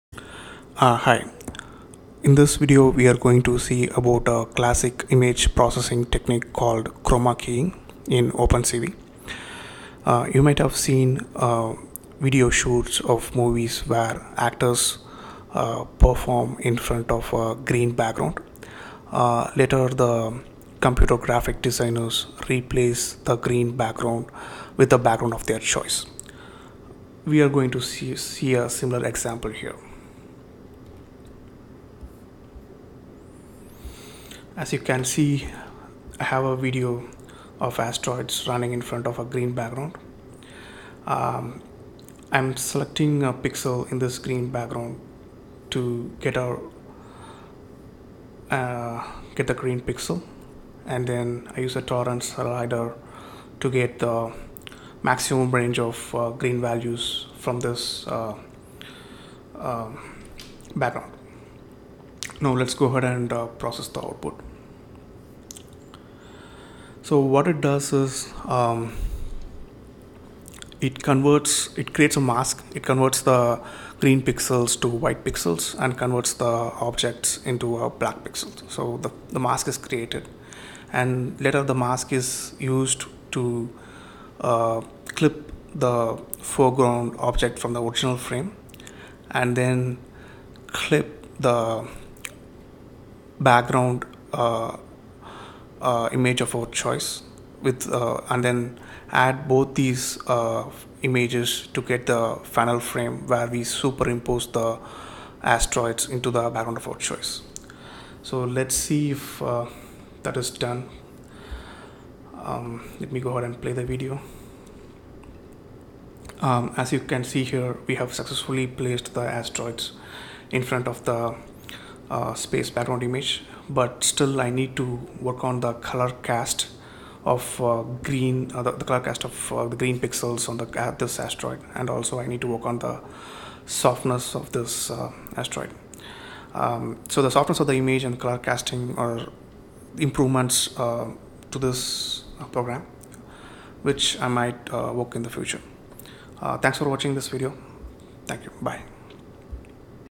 Chroma Keying in OpenCV
Chroma Keying in OpenCV - Convert the green background to the background of your choice